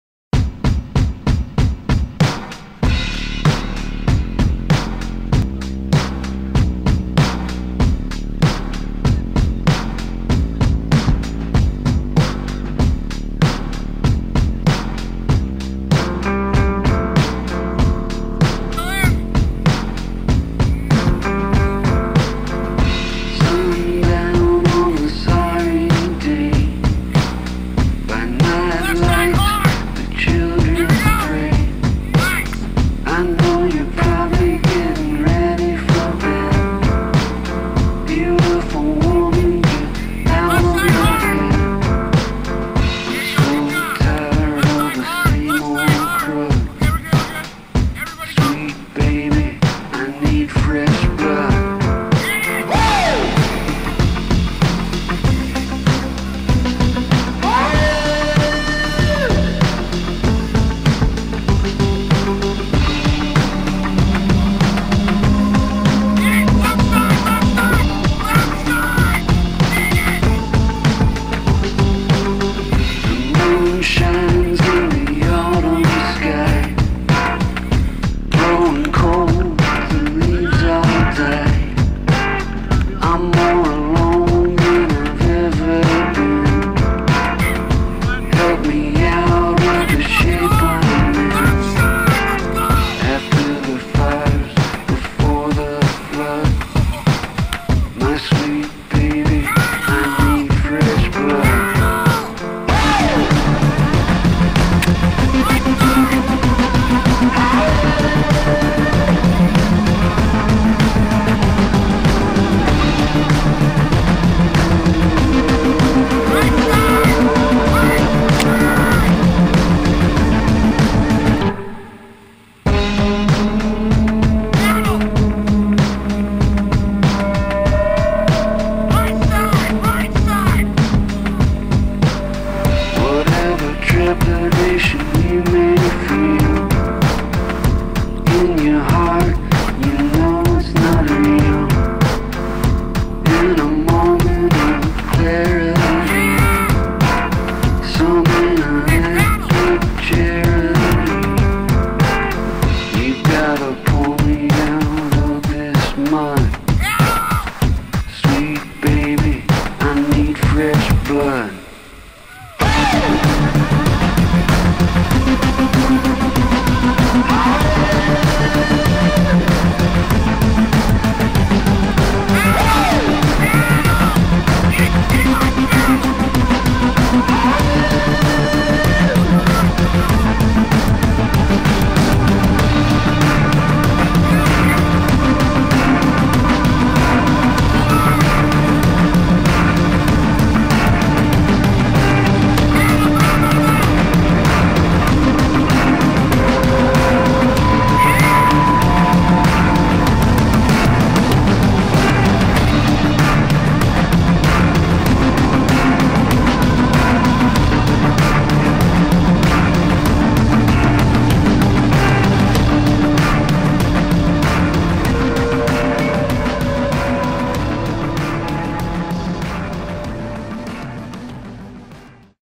Lochsa 2015
Whitewater Rafting May 23, 2015 on the Lochsa River, ID